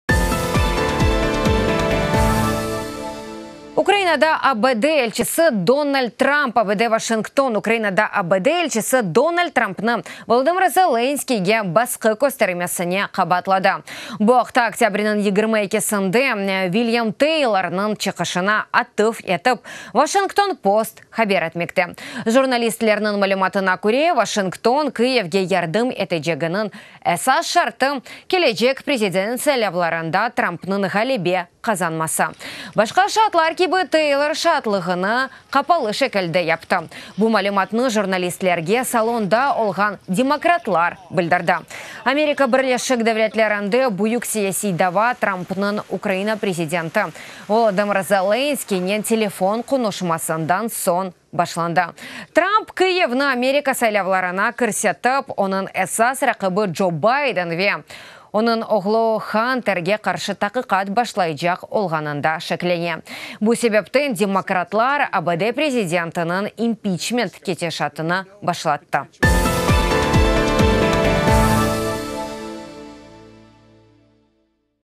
TRUMP QABAATLANDI
Ukrainada ABD Elçisi Donald Trampnı Volodımır Zelenskiyge basqı köstermesinde qabaatladı. Bu aqta oktâbrniñ 22-nde Vilyam TEylornıñ çıqışına atıf etip, Vaşington Post haber etmekte.